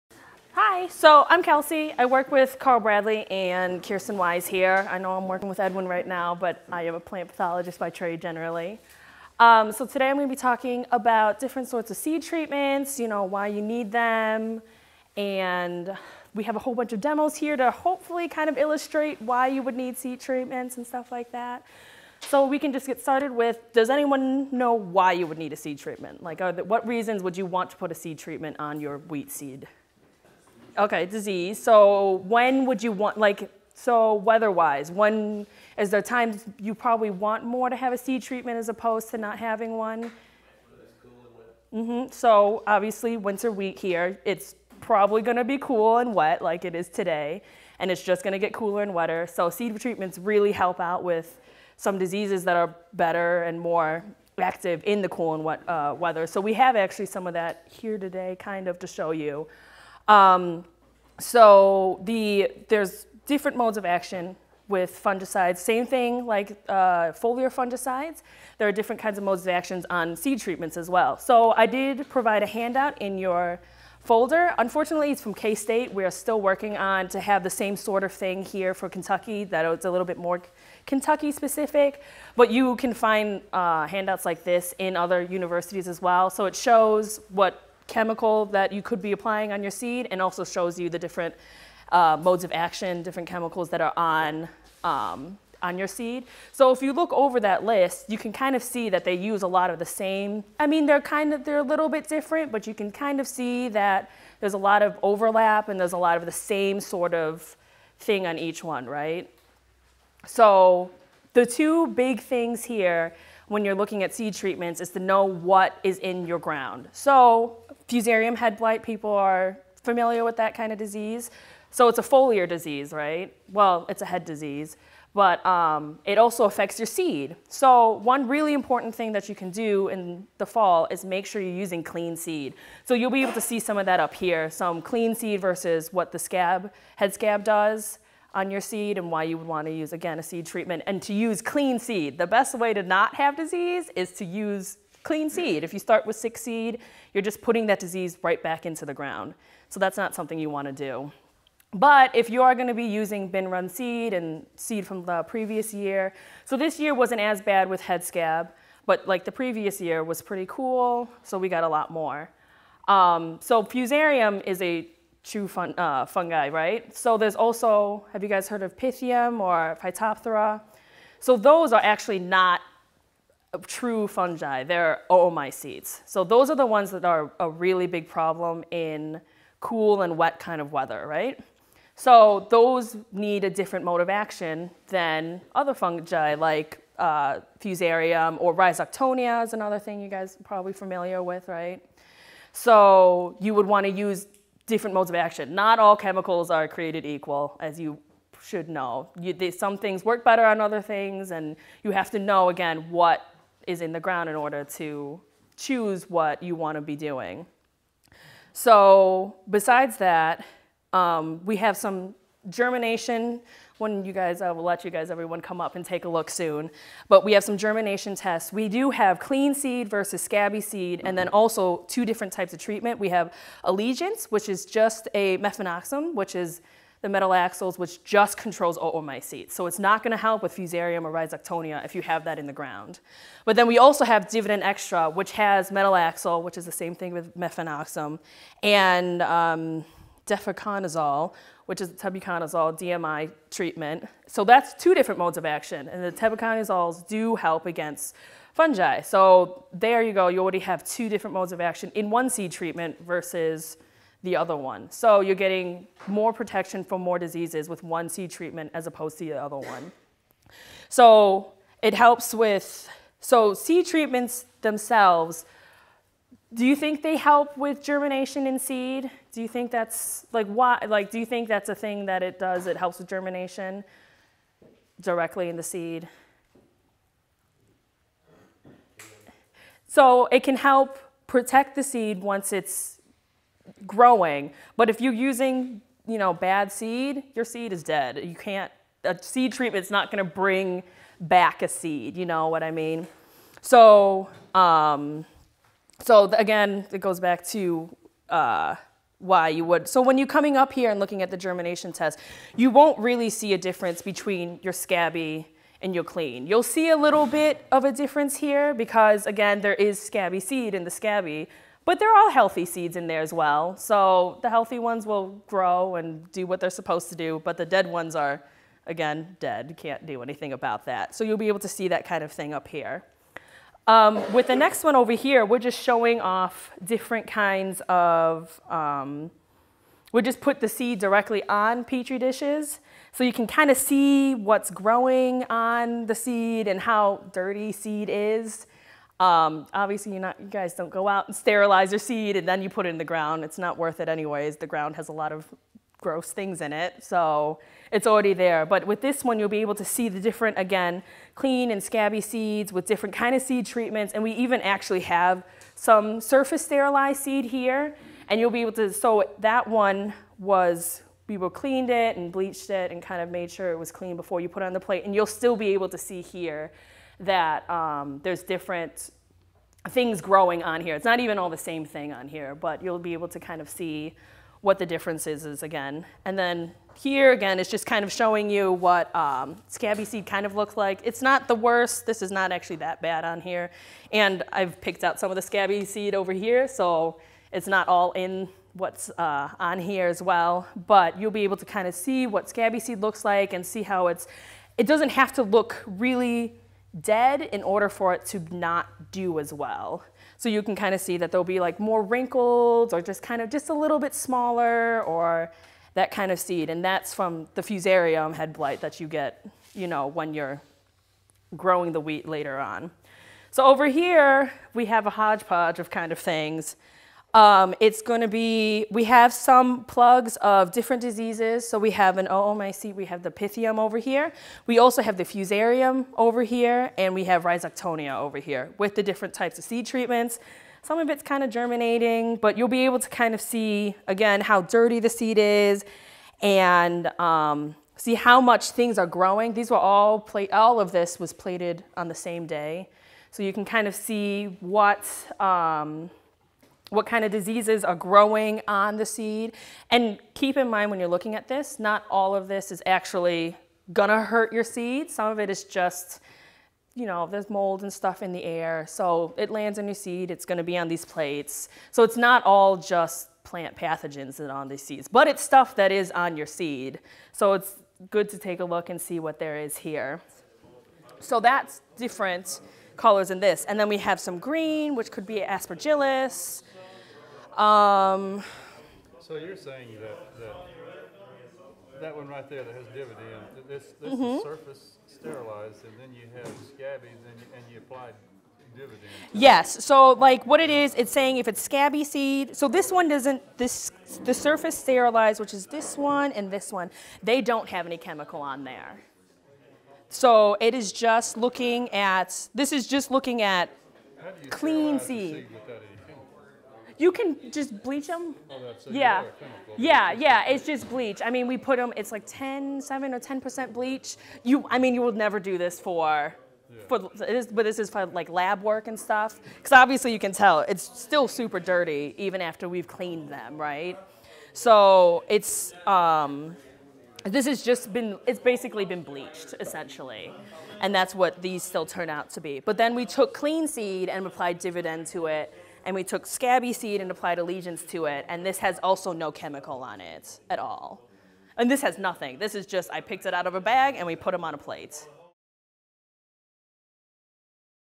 Fungicide Seed Treatments
at the 2017 Wheat Field School pre plant, plant pathologist Kelsey Mehl spoke on fungicide seed treatments